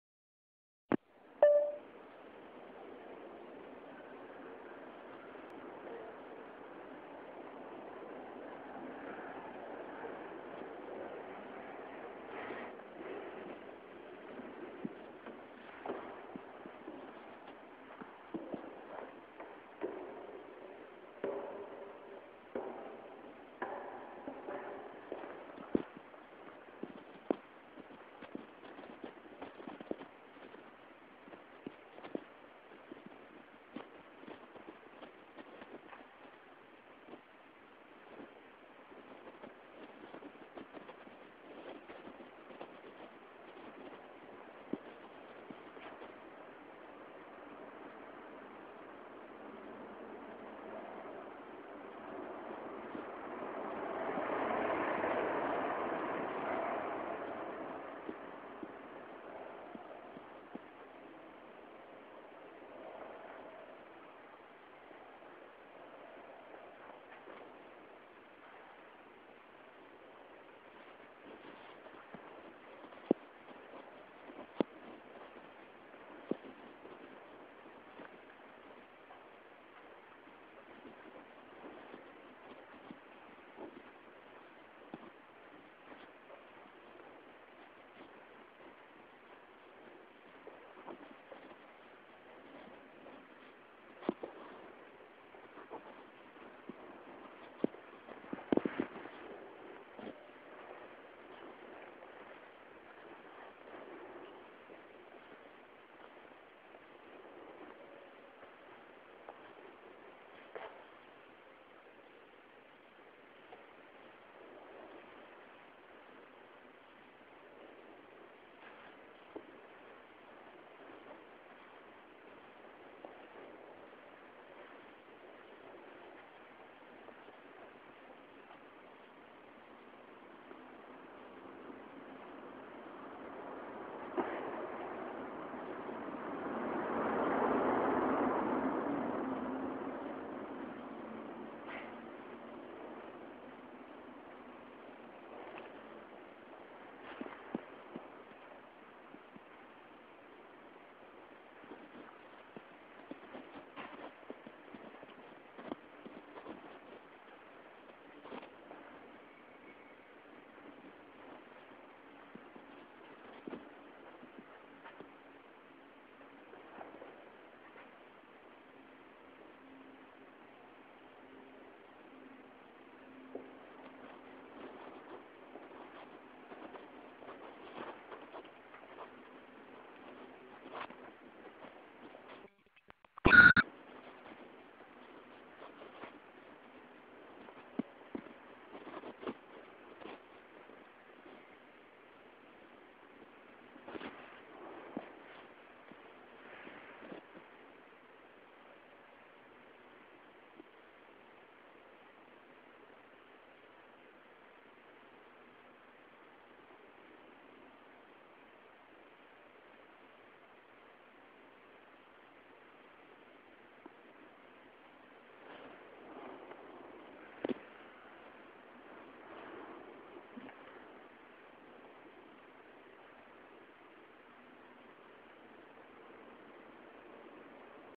Sinfonia di colori capua 2012
Mostra di pittura itinerante di salvatore nardelli